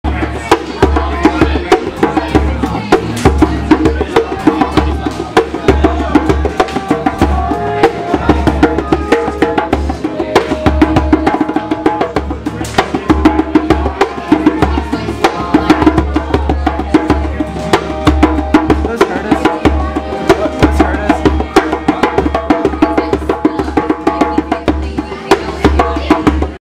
Get Back Day Rhythms: Part 1
It's get back day at the WT property, and while some spend their down time playing four square, others decide to entertain everyone in the lodge by making some rhythmic music.